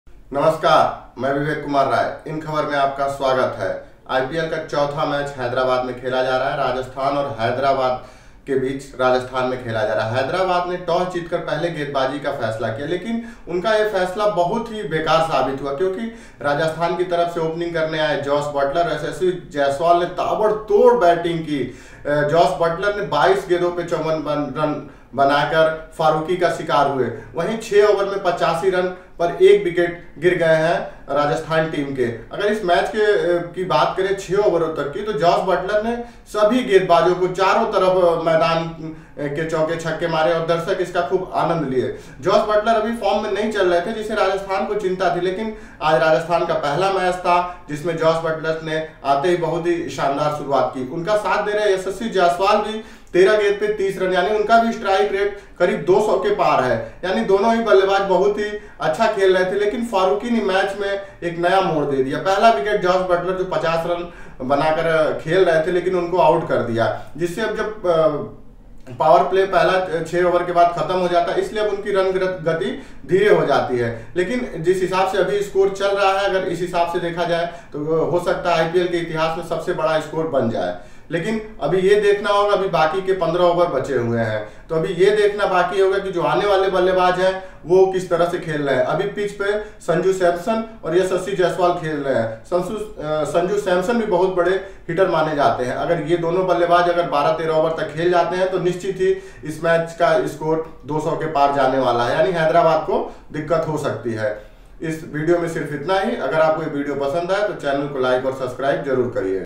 SRH vs RR Live Score: Jos Buttler की तूफानी पारी 250 के स्ट्राइक रेट बनाए 54 रन, राजस्थान की जीत तय?
SRH vs RR Live Score: Jos Buttler की तूफानी पारी 250 के स्ट्राइक रेट बनाए 54 रन, राजस्थान की जीत तय? देखें वीडियो..